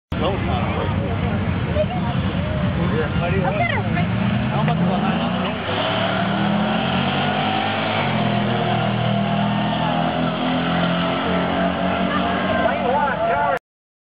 Eclipse GSX 1/8 mile
vs truck with Chevy 350 V8